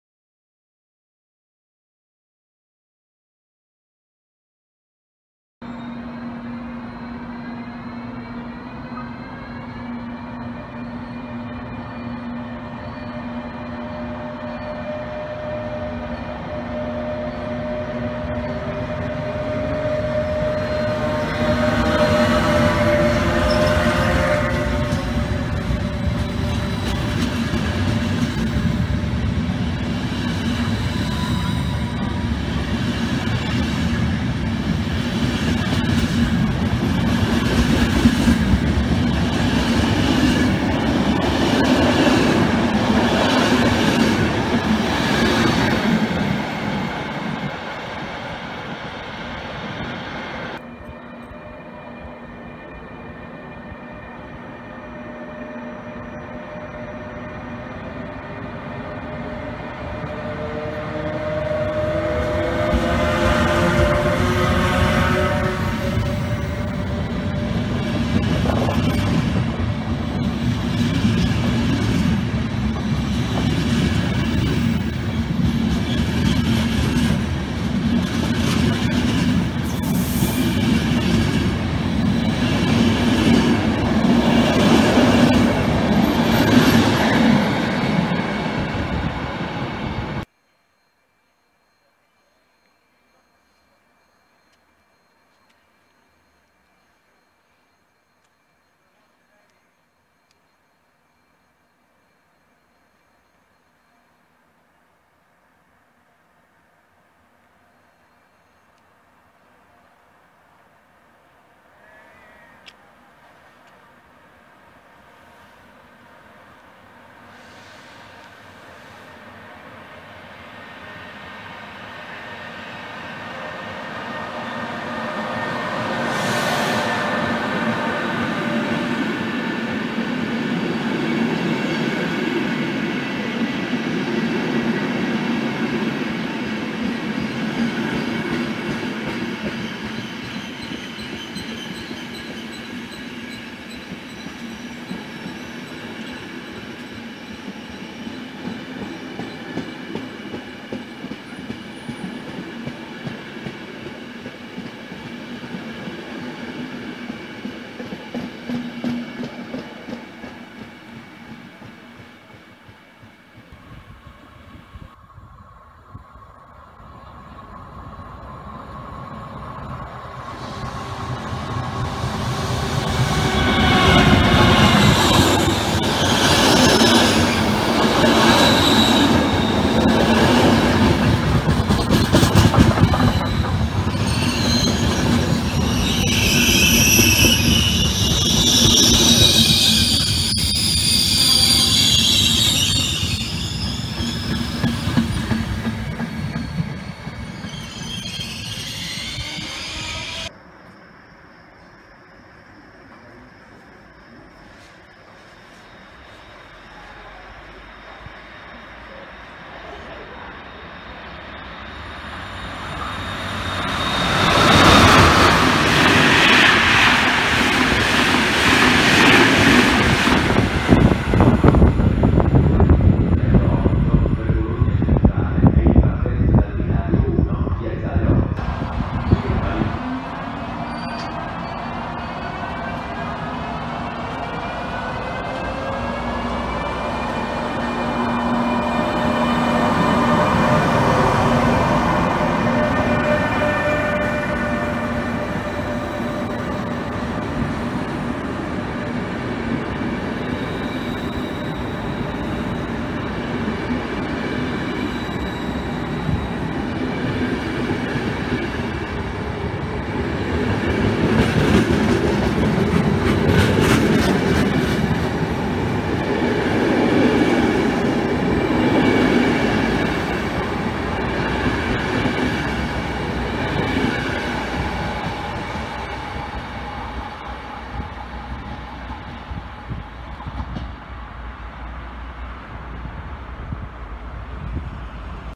UN VECCHIO RICORDO DELLE E402B SUI FRECCIABIANCA ADRIATICI...
Ed eccoci in nuovo video, sta volta andando a ripescare le E402B quando ancora erano a capo dei Frecciabianca Adriatici.Fino al 2017 era più che normale vederle in servizio,ma con l'entrata in servizio delle E414 le E402B sono passate a svolgere gli Intercity.Ai giorni d'oggi sono rimaste in livrea Frecciabianca(senza logo) le seguenti unità: 103,110,114,124,136,138,162,164,165,166,171,176 e 178.Un eccezione è stato il 5 Settembre 2019 dove il Frecciabianca 8888 Lecce-Venezia S.L. è stato straordinariamente effettuato con la E402B.151 e il giorno dopo il Frecciabianca 8815 Venezia S.L.-Lecce è stato effettuato con la E402B.179 nella nuova livrea IC SUN e da quel giorno non si sono più visti Frecciabianca snavettati.In questo video potrete vedere vari filmati registrati tra le stazioni di Imola e Pesaro di alcuni Frecciabianca effettuati con E402B XMPR,FB e IC SUN.

Nell'ordine tutti i treni che avete visto:

)FB 9811 Milano C.le-Lecce | E402B.104 PAX-LH
)FB 9813 Milano C.le-Lecce | E402B.126 PAX-LH
)FB 9826 Lecce-Milano C.le | E402B.126 PAX-LH
)FB 9880 Lecce-Milano C.le | E402B.128 PAX-LH
)FB 8888 Lecce-Venezia S.L. | E402B.151 PAX-LH
)FB 8815 Venezia S.L.-Lecce | E402B.179 PAX-LH